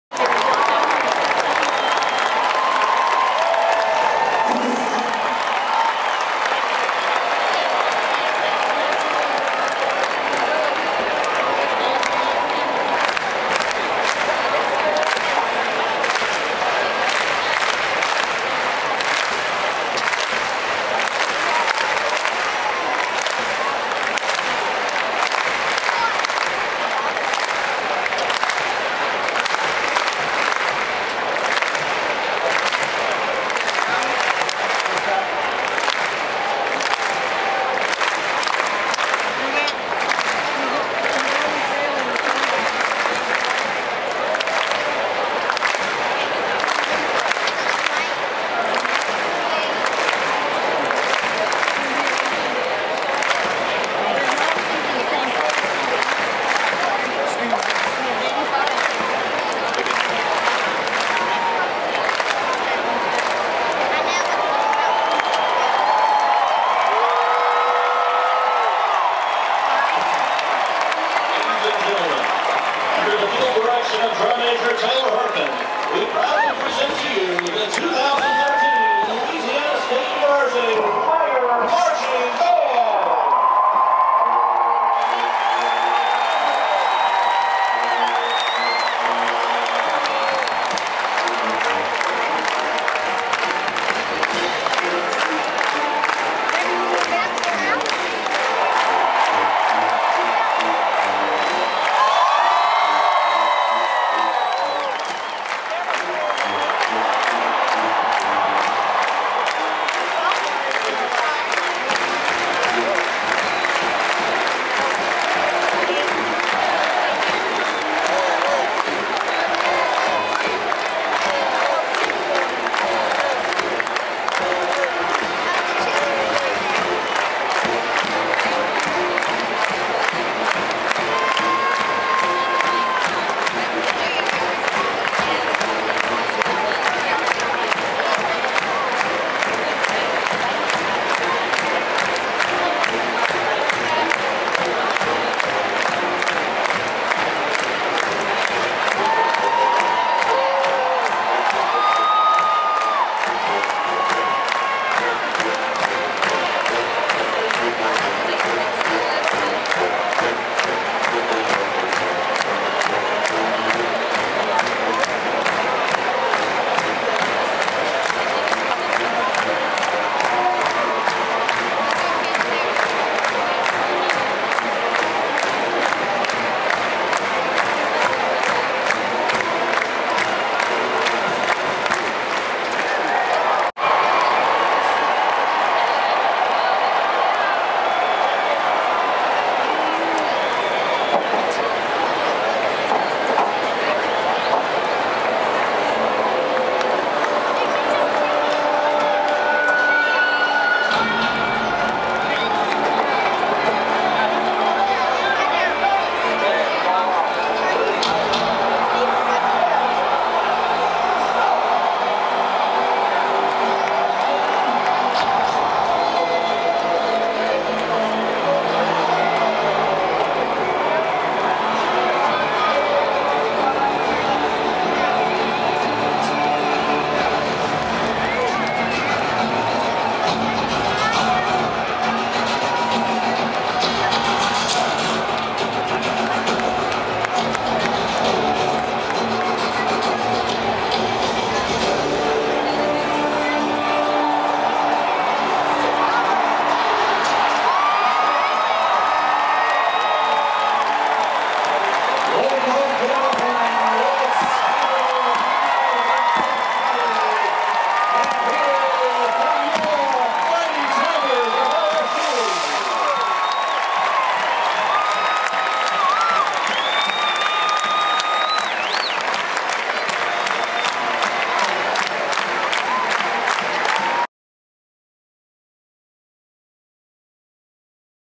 Golden Band from Tigerland Pre-Game (9-14-13) Baton Rouge, Louisiana US~LSU 45 Kent State 13 (HD)
The Golden Band from Tigerland performs the pre-game on Saturday 9-14-13 before the #8 ranked LSU Tigers took on Kent State and beat them 45-13.